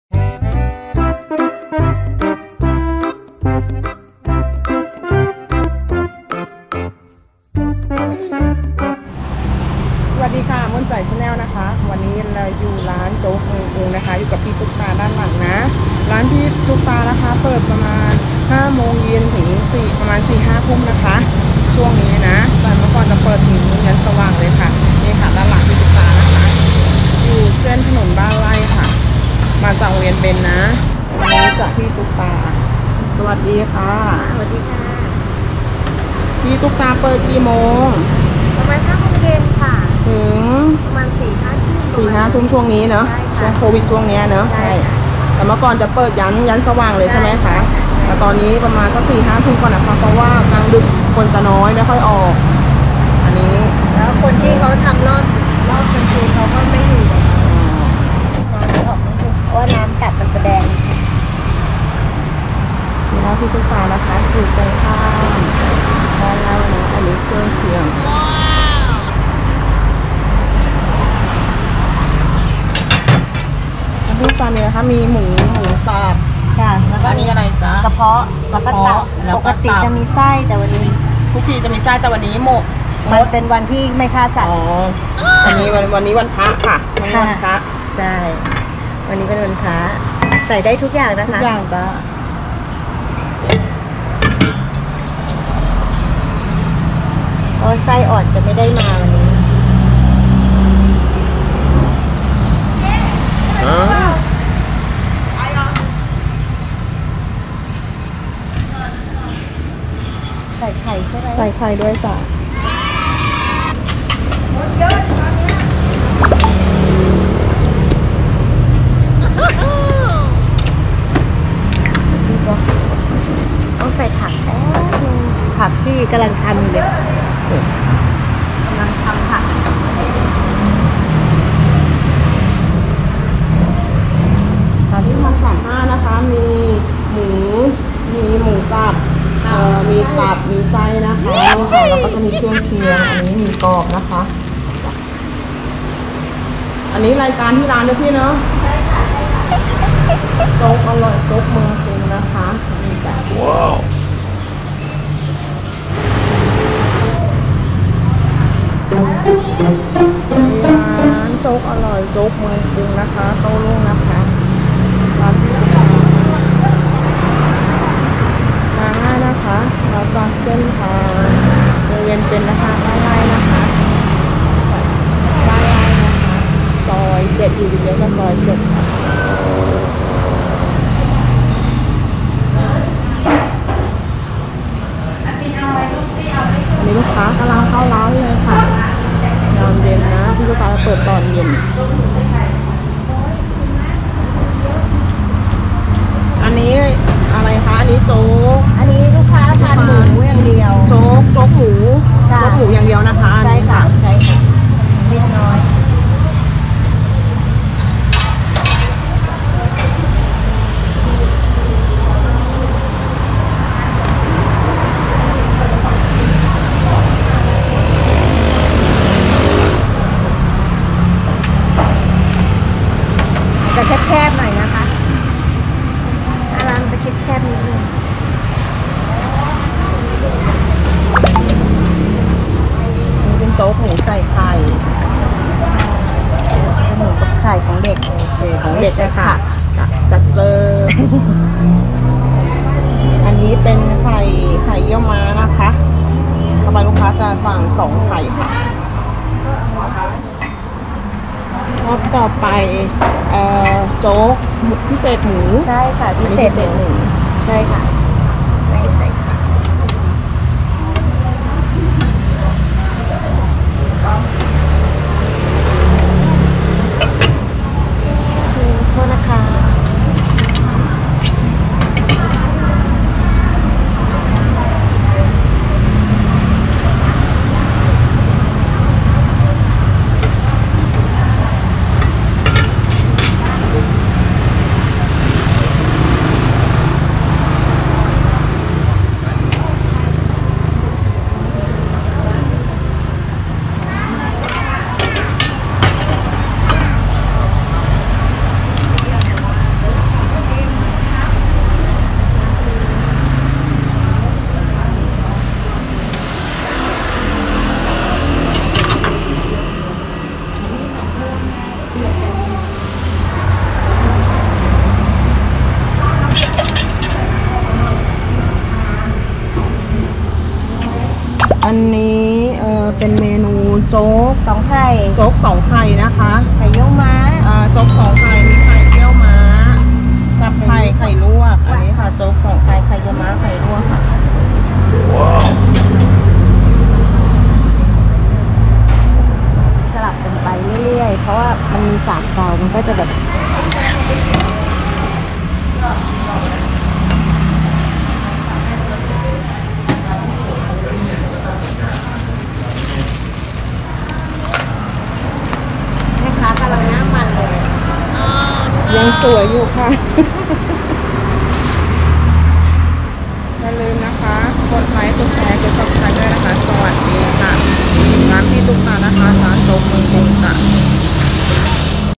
โจ๊กเมืองกรุง โจ๊กสองไข่ เครื่องแน่นเน้นไข่/ร้านอร่อยราชบุรี
ม่วนใจ๋พาชิม ร้านโจ๊กเมืองกรุง ราชบุรี
ร้านตั้งอยู่ที่บ้านไร่ เยื้องบ้านไร่ซอย 7 เลยโรงเรียนเบญไปไม่ไกล ร้านนี้เปิดตั้งแต่ 5 โมงเย็น ถึง 5 ทุ่ม เมนูแนะนำจะเป็น โจ๊กสองไข่ น่าทานมาก ลองมาชิมกันดูจ้า
ตืดตามม่วนใจ๋อีกได้ที่